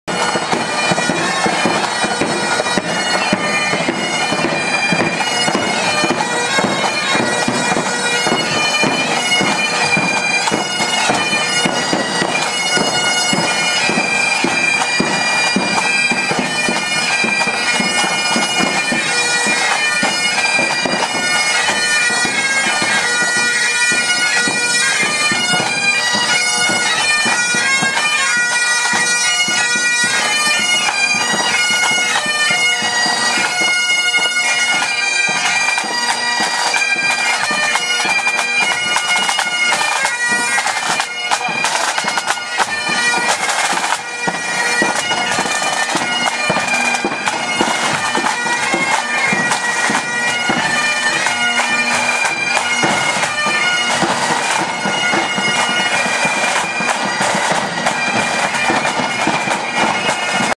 White Water Pipe Band @ South Down Defenders FB Parade 2011
White Water Pipe Band @ South Down Defenders Flute Band Parade Friday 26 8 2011 In Loyalist Newry

Support The Loyalist Band Scene.
Sticks & Stones Will Break Your Bones But Flutes Will Never Harm You